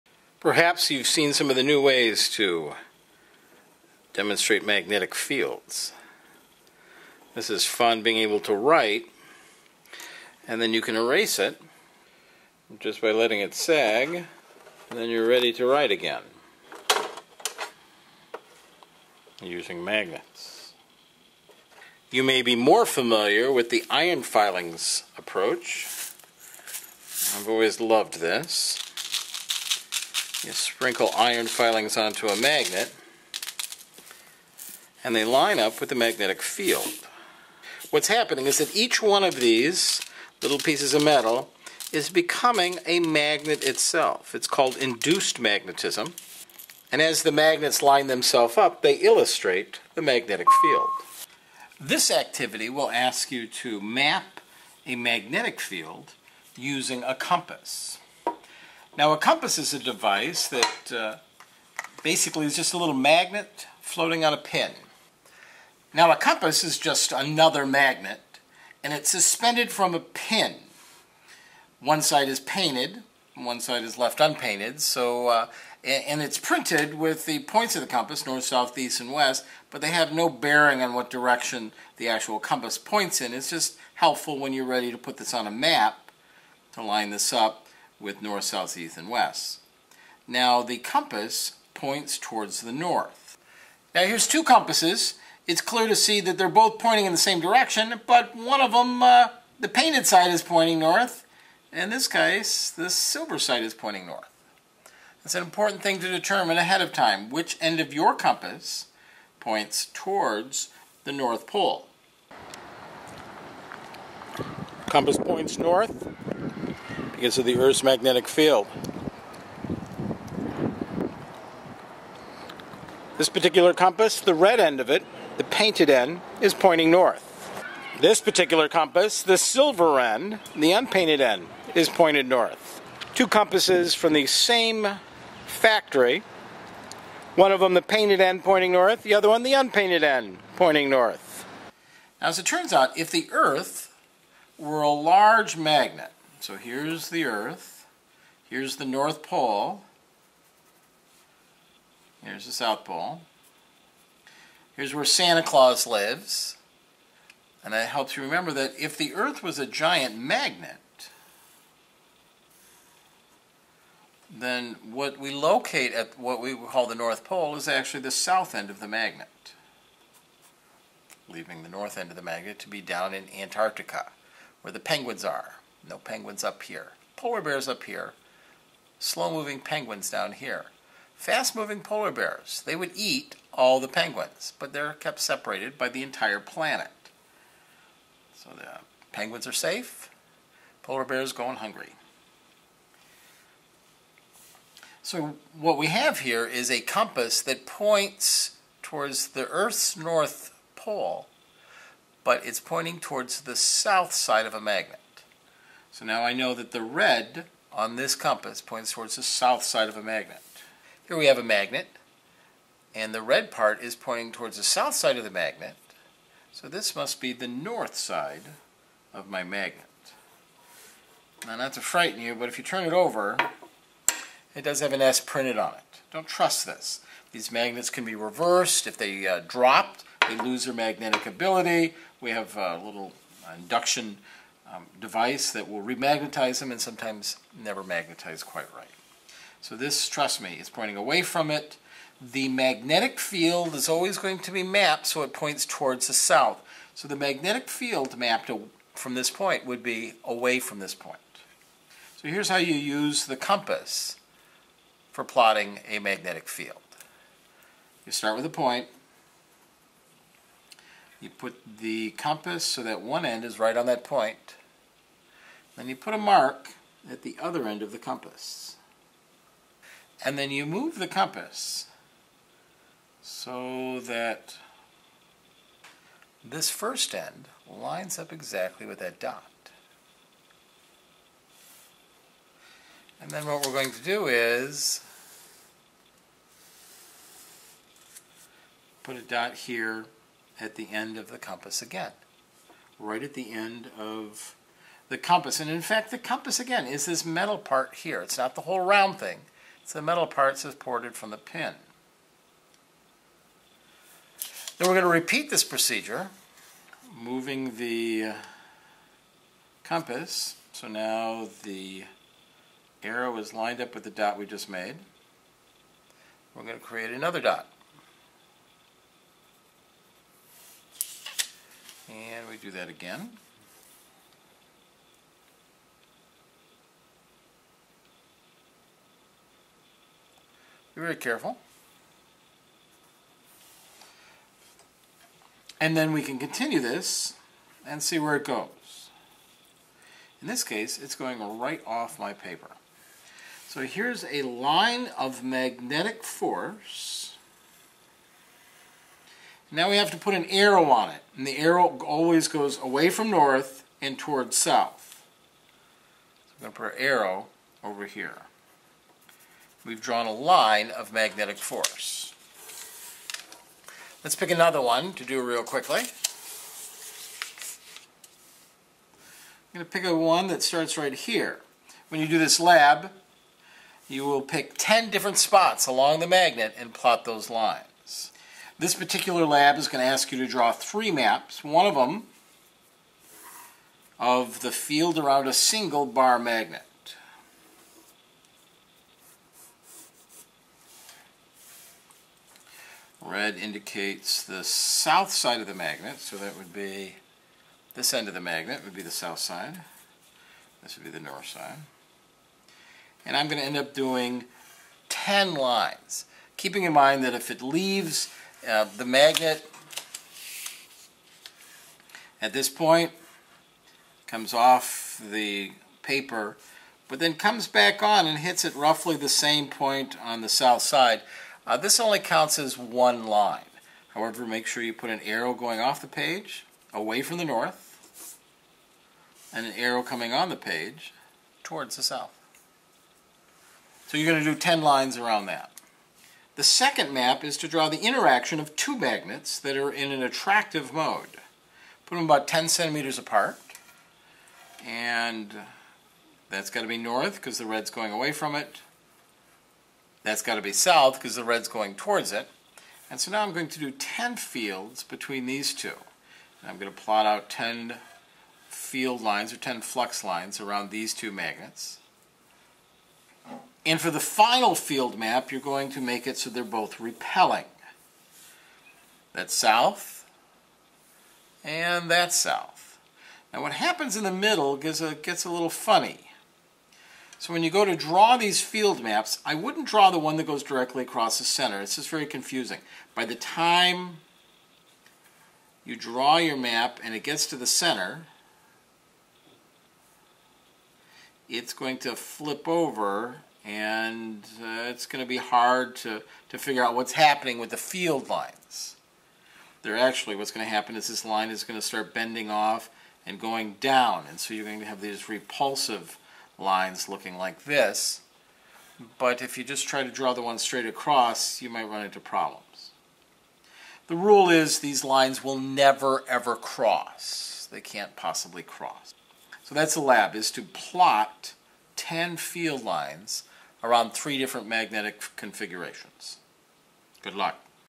Mapping a magnetic field
Intro to a high school physics lab using a compass to map a magnetic field.